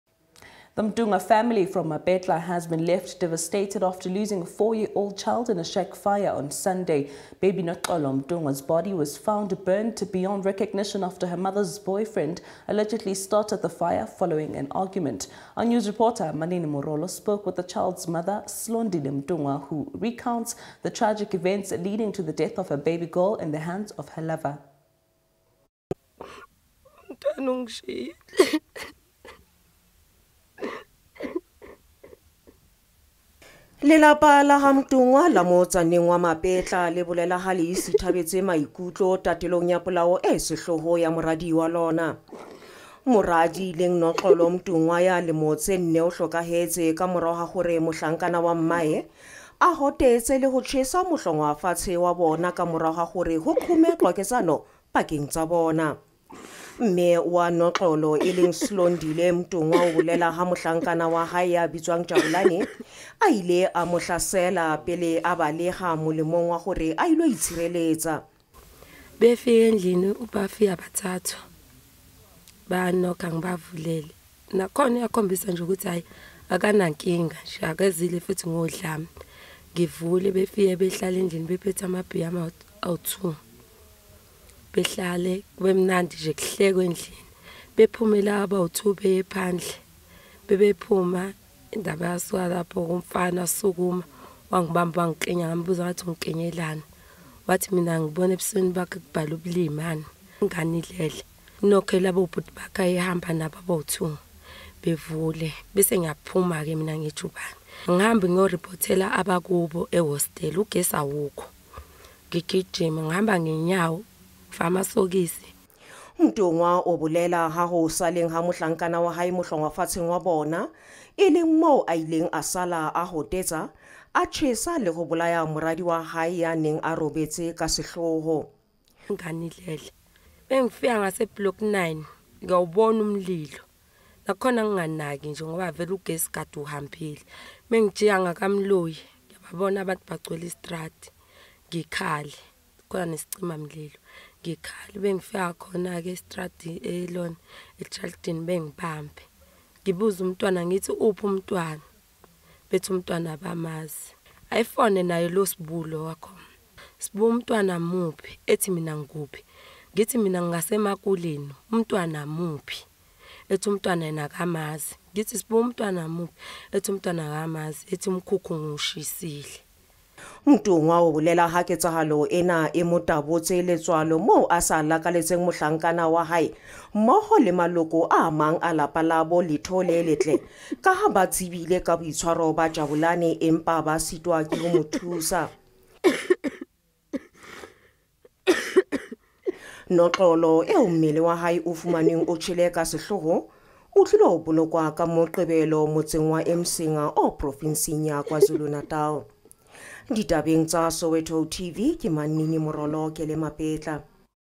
SOWETO TV NEWS | MOTHER FROM MAPETLA DEVASTATED AFTER LOSING A CHILD IN A FIRE STARTED BY BOYFRIEND
A mother from Mapetla has been left devastated after losing a four-year-old child in a shack fire allegedly started by her boyfriend following an argument.